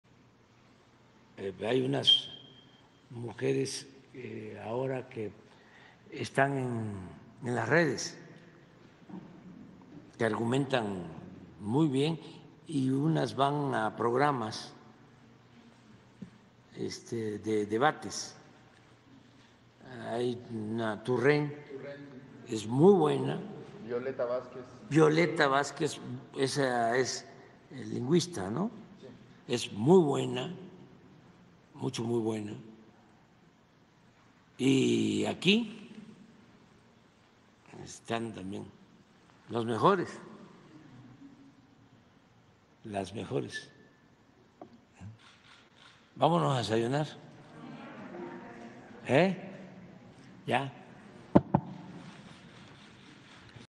AMLO RECONOCE ANÁLISIS Y REFLEXIONES DE NUESTRA COMPAÑERA RENATA TURRENT
El presidente López Obrador reconoció el análisis y reflexiones de nuestra compañera Renata Turrent, al contrarrestar las calumnias contra su gobierno en los medios corporativos.